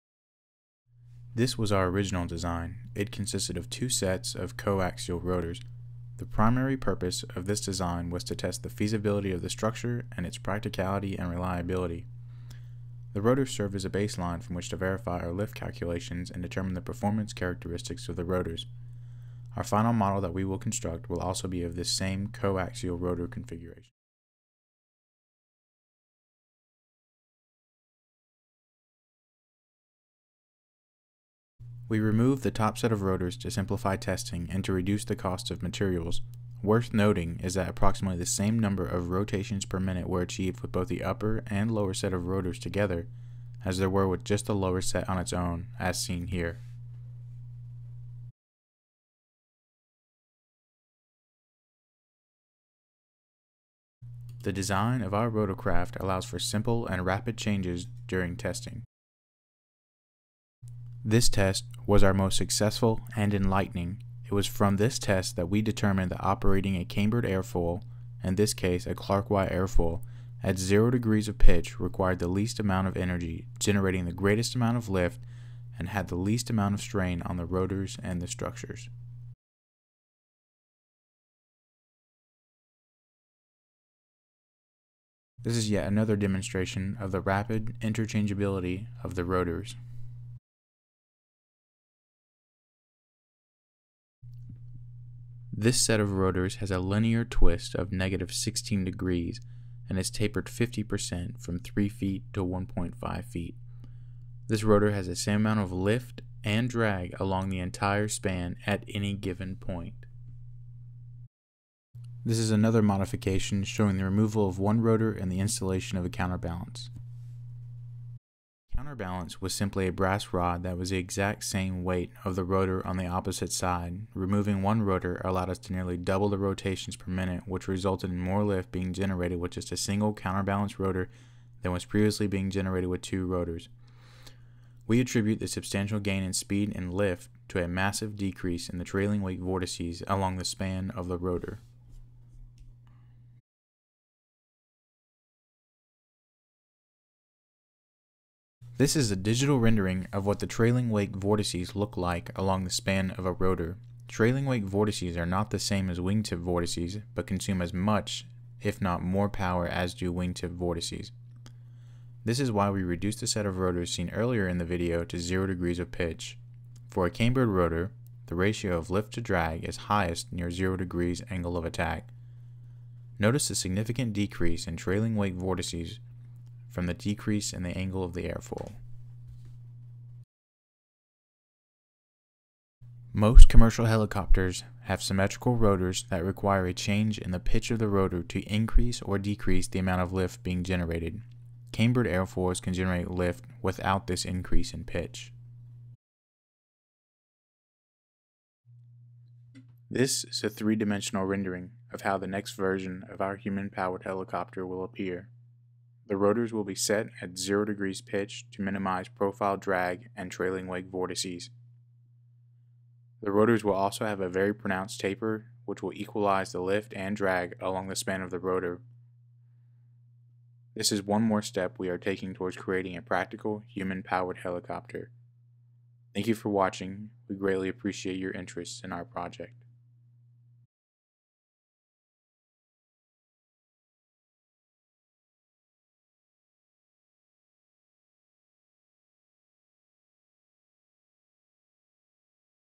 Human Powered Helicopter Ground Tests
Human Powered Helicopter Project Ground Tests

This is a short video summary of the work done at Middle Georgia State University from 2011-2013. The total cost of all of the work and prototypes show was only $7,500.

This Project concluded in 2013 without the final design being constructed and tested due to a lack of funding from the University after the conclusion of the Sikorsky Competition.

The development of the optimized rotor blades are in the process of being implemented on a electric ultralight helicopter as of 2019